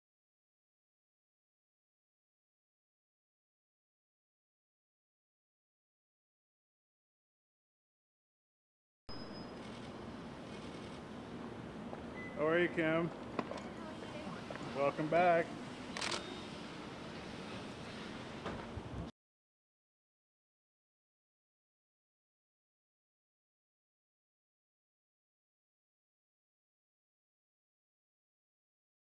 KYM JOHNSON after DAVID ARQUETTE Sneaks Out The Back DWTS S1470
S1470 STEVE BRODERSEN REPORTS 082511
 Kym Johnson - reported for duty at Dwts rehearsals.
She met w/ rumored partner David Arquette, who sneaked out the back.
Guess they're keeping things top secret until the official announcement on Monday.